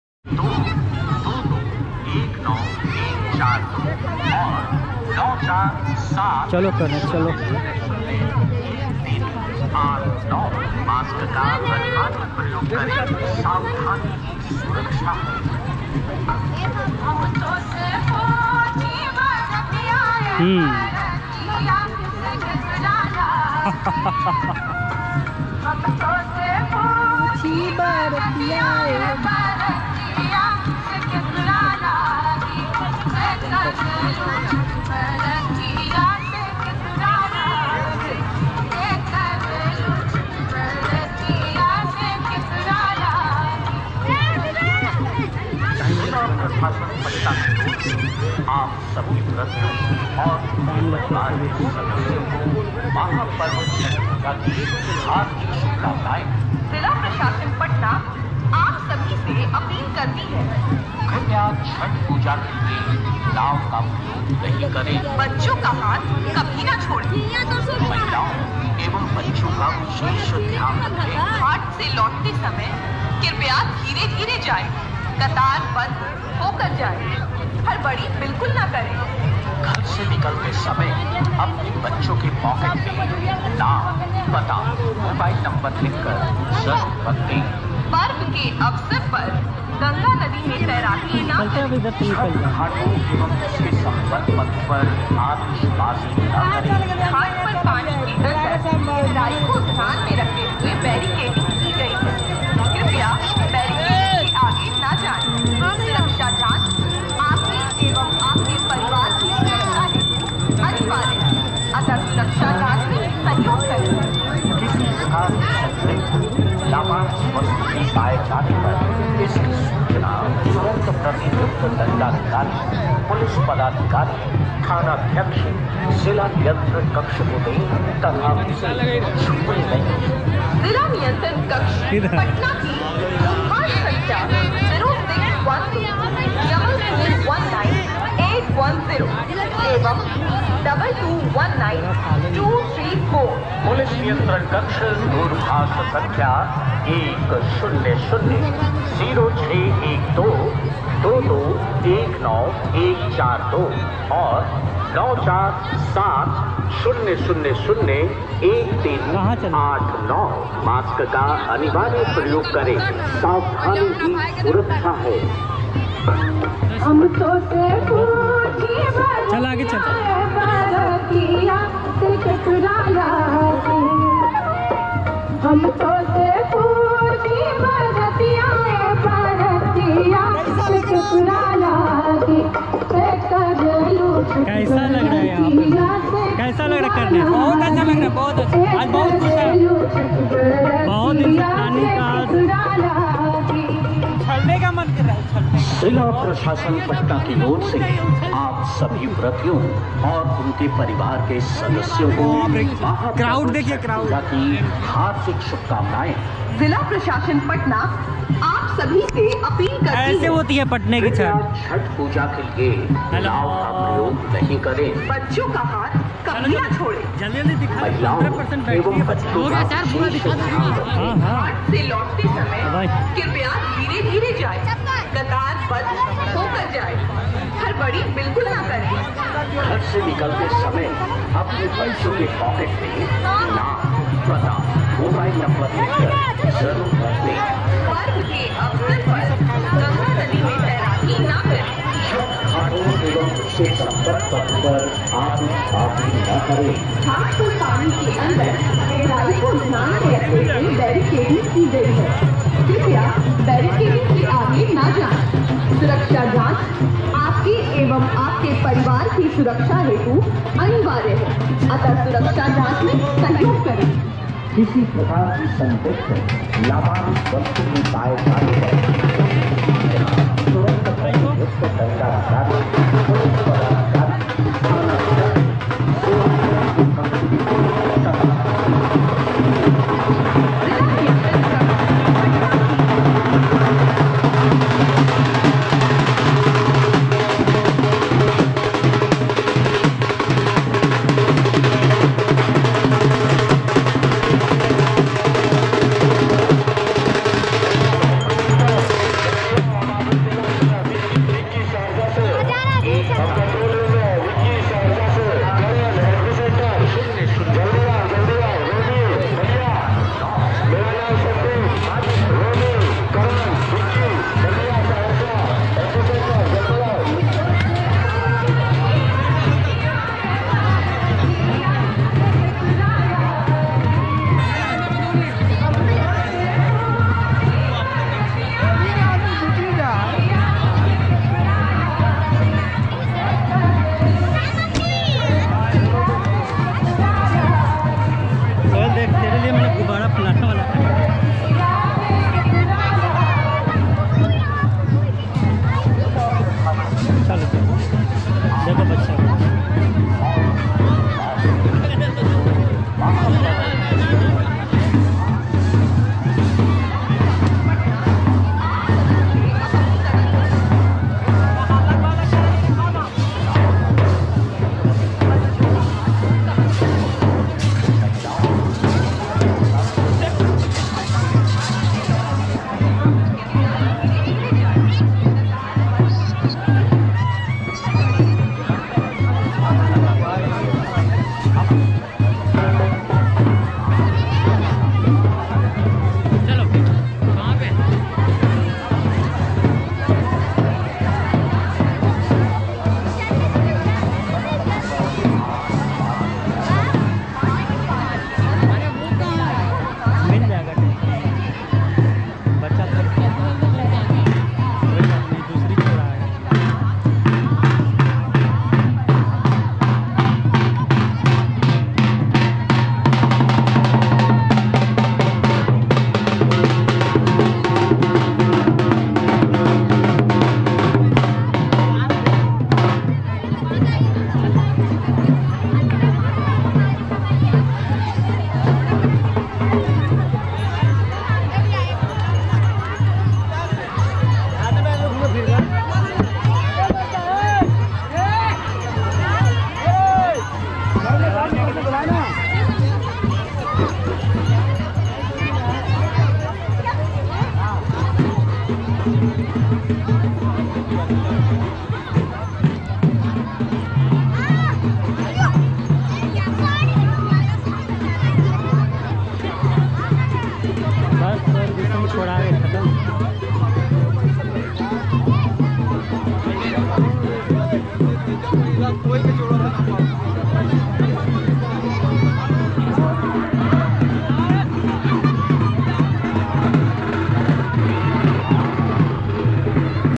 चैती छठ पूजा 2022 लाइव अपडेट पटना के घाट से, CHHATH POOJA SPECIAL VLOG @Wedding travelers,
My first Chhath Pooja in Patna. this place is located in behind the BN collage ghat.

लोक आस्था का महापर्व छठ भगवान सूर्य को अर्घ्य देने के साथ ही संपन्न हो गया. आज चौथा और अंतिम दिन था. आज सुबह करीब 05 बजकर 47 मिनट पर सूर्योदय होने के साथ ही अर्घ्यदान का क्रम आरंभ हो गया था. इसके बाद व्रती व उनके स्वजनों ने भगवान भास्कर को अर्घ्य देकर खुद के लिए और समाज व देश के हित की कामना की.इससे पहले शुक्रवार की अलसुबह से ही श्रद्धालु पास के छठ घाटों पर पहुंचने लगे थे. इन घाटों पर रोशनी की बेहतर व्यवस्था होने से यहां का दृश्य मनोहारी था.